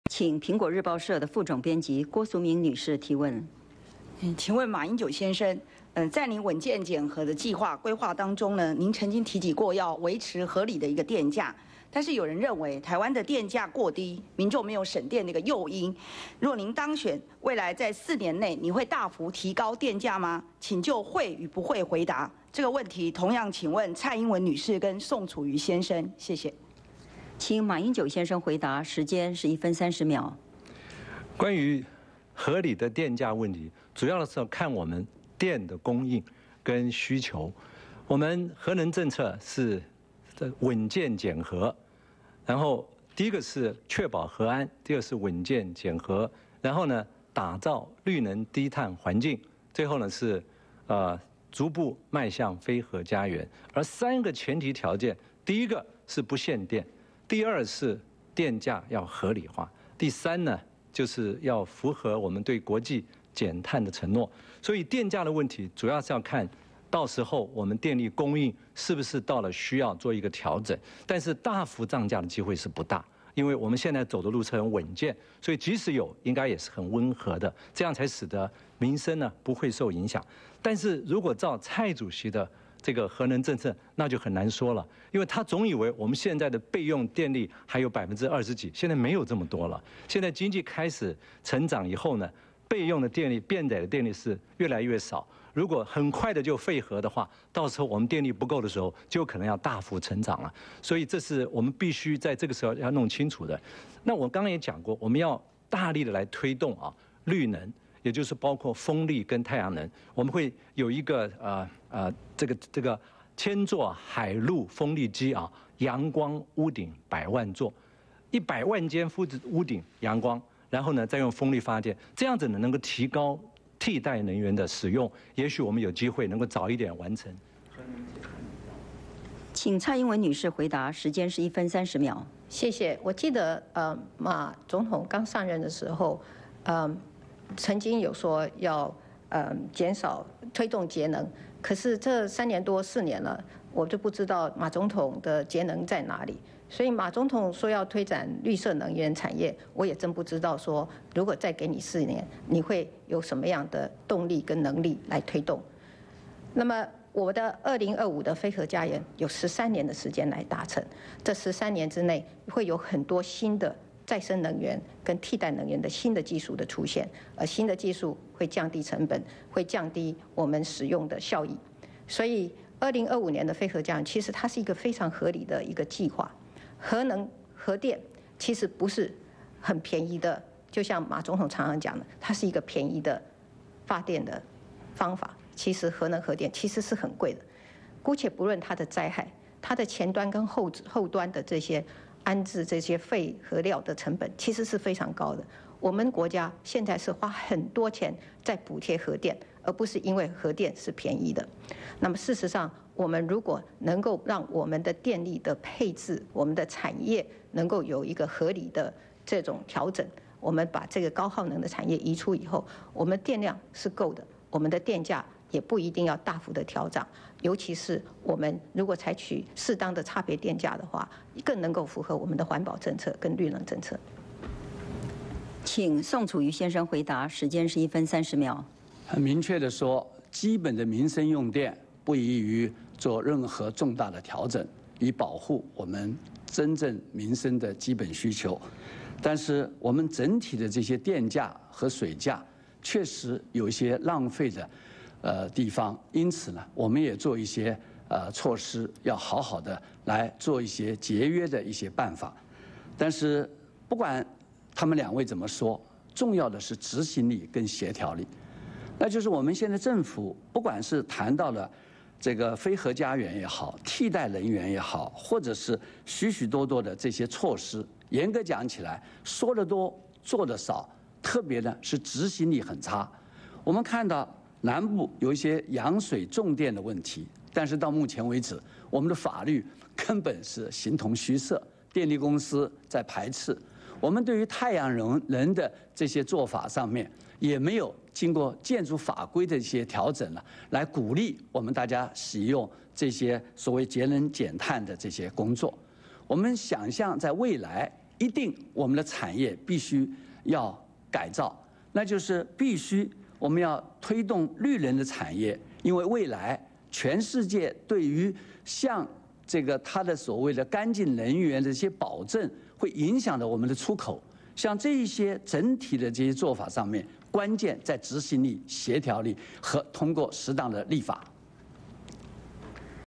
第二階段 媒體提問 蘋果日報副總編輯 郭淑敏 提問7
蘋果日報副總編輯 郭淑敏 提問7
馬英九先生回答(1分30秒)   蔡英文女士回答(1分30秒)   宋楚瑜先生回答(1分30秒)

蘋果日報副總編輯郭淑敏提問：台灣能源危機，電價會大幅調高嘛？
馬英九答：我的政策是確保核安、穩健減核，逐步邁向非核家園，三大重點是不限電、電價合理化、符合對國際減碳的承諾。大幅調整的機率不大，而是穩健。備用電力沒有蔡主席那麼多，很快廢核卻會造成電力危機。大力推動綠能，風力和太陽能，預計打造陽光屋頂百萬座。

蔡英文答：馬總統上任三年多，都看不到他做了什麼綠能措施，我的2025非核家園，有13年來實現，期間會有綠能發展，核能核電不是很便宜，並非馬總統所說的便宜能源，姑且不論他的災害，前後端安置廢料要花很多費用，所以我們是花很多錢在補貼核電，不用大幅調整，採取適當差別電價更能符合環保政策。

宋楚瑜答：民生用電不該大幅調整以免影響民生，電價、水價確實不合理的便宜，所以要有所調整，不管他們兩位怎麼說，重要的是執行力和協調力。我們的法律是形同虛設，對於太陽能的作法也沒有透過建築法規的調整去促進，我們想像在未來產業要改造，推動綠能產業。